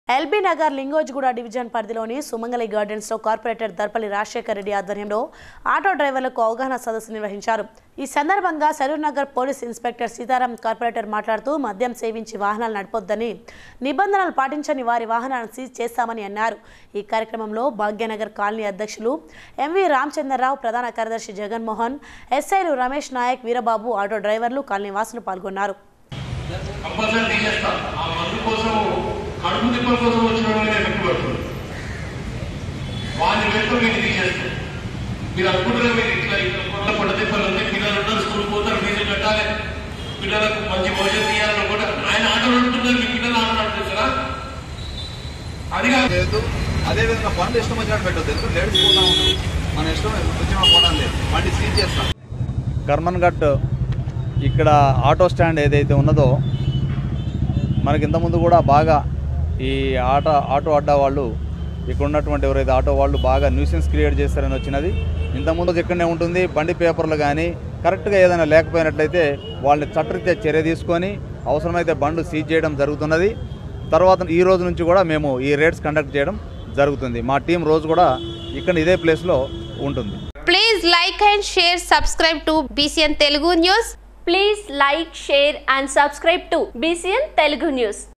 ఆటో డ్రైవర్లకు అవగాహన సదస్సు నిర్వహించిన కార్పొరేటర్ దర్పల్లి రాజశేఖరరెడ్డి
ఎల్బీనగర్ , లింగోజిగూడ డివిజన్ పరిధిలోని సుమంగళి గార్డెన్స్ లో  కార్పొరేటర్ దర్పల్లి రాజశేఖరరెడ్డి ఆధ్వర్యంలోఆటో డ్రైవర్లకు అవగాహన సదస్సు నిర్వహించారు. ఈ సందర్భంగా సరూర్‌నగర్‌ పోలీసు  ఇన్‌స్పెక్టర్ సీతారామ్, కార్పొరేటర్ మాట్లాడుతూ మద్యం సేవించి వాహనాలను నడపొద్దని, నిబంధనలు పాటించని వారి వాహనాలను సీజ్ చేస్తామని అన్నారు. ఈ కార్యక్రమంలో భాగ్యనగర్ కాలనీ అధ్యక్షులు ఎంవీ రాంచందర్ రావు, ప్రధాన కార్యదర్శి జగన్మోహన్, , ఎస్ ఐలు రమేష్ నాయక్, వీరబాబు, ఆటో డ్రైవర్లు, కాలనీవాసులు పాల్గొన్నారు.

An awareness conference was held for auto drivers under the leadership of Corporator Darpalli Rajasekhara Reddy in Sumangali Gardens under Lingojiguda Division, LB Nagar. On this occasion Sarurnagar Police Inspector Sitaram, Corporator said that they should not drive vehicles under the influence of alcohol and the vehicles of those who do not follow the rules will be impounded. Bhagyanagar Colony President MV Ramchander Rao, General Secretary Jaganmohan, SIs Ramesh Naik, Veera Babu, auto drivers and colony residents participated in this program.